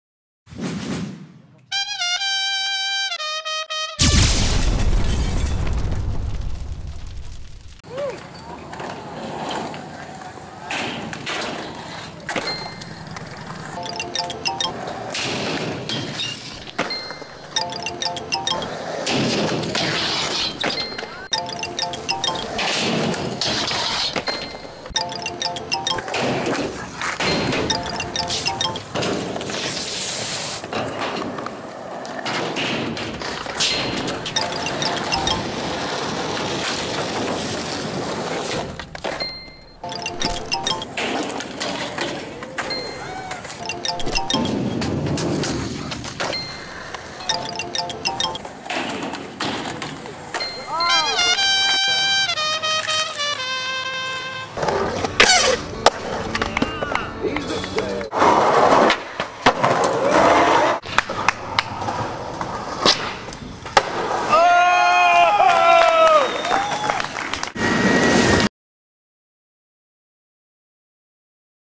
Brandon Moore - 10 Tricks For A Taco
Brandon Moore gives this prefab park a run for its money.
Taco Fillin:
1. Nosegrind 180 out
2. Back Lipslide
3. Backside Lipslide
4. Frontside Smith Grind
5. Tre Flip
6. Frontside Flip
7. Switch Frontside Bigspin
8. Crooked Grind Back Lipslide
9. Hardflip
10. Feeble Grind Back Lipslide